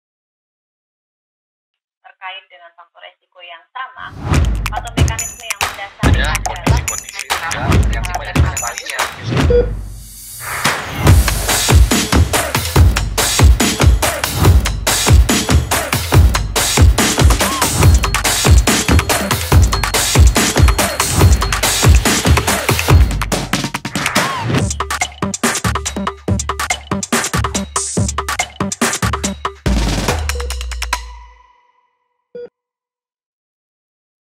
8th Annual Scientific Meeting InaHRS 2021
Scientific Meeting Indonesian Heart Rhythm Society (InaHRS) 2021, which will be held from September to December 2021.

This event will be divided into 2 online session; webinar series sessions and scientific sessions